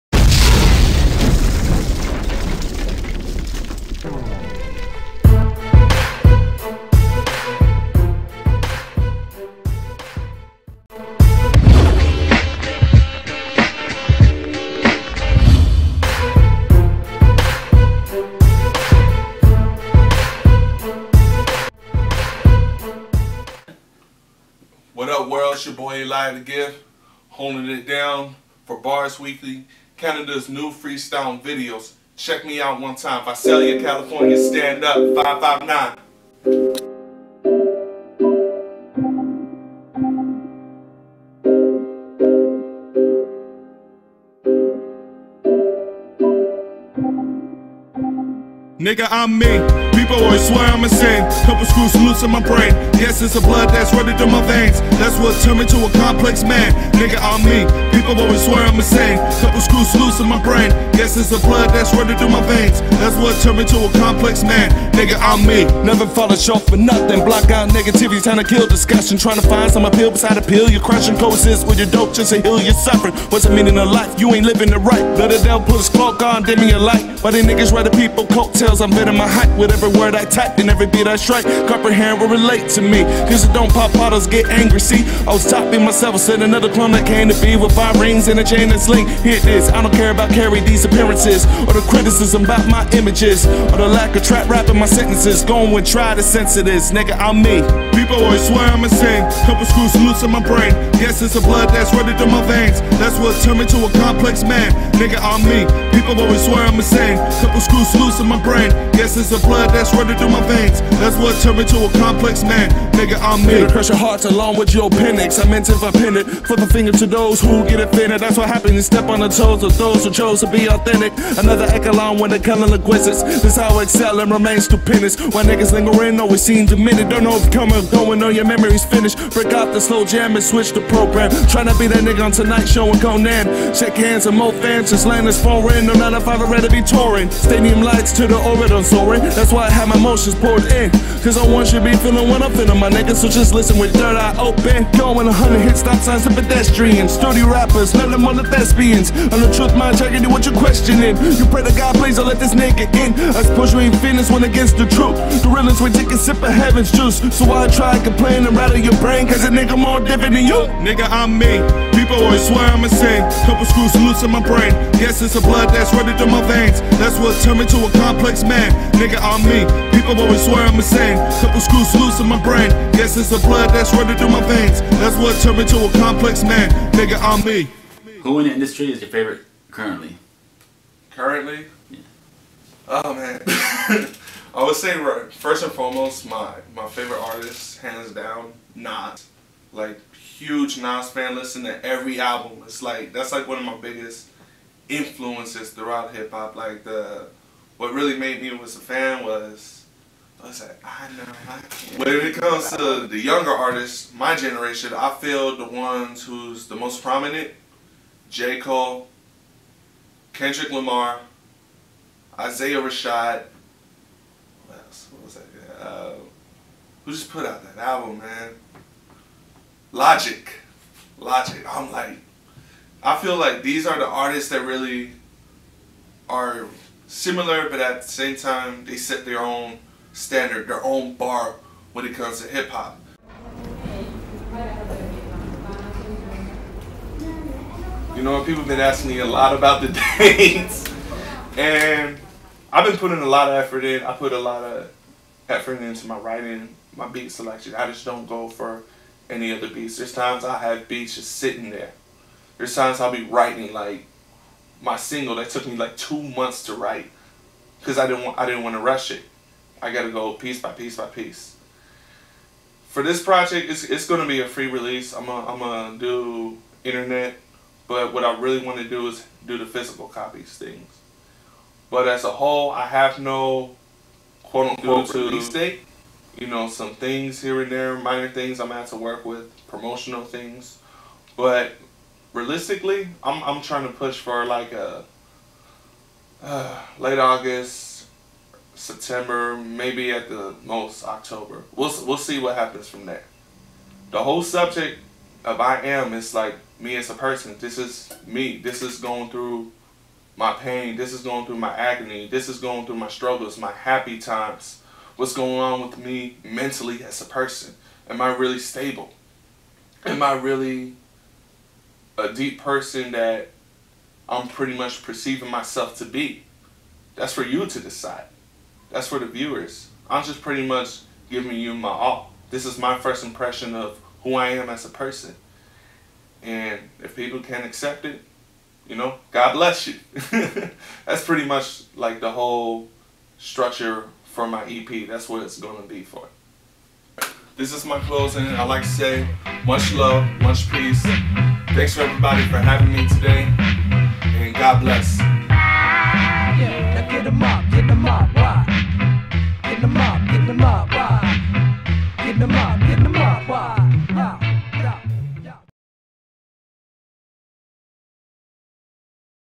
Bars weekly Episode 7 Eliah The Gift
one of my favourite new artists out of the rockstargimmedat camp
from California "Eliah the Gift" ..
Eliah the Gift on Youtube channel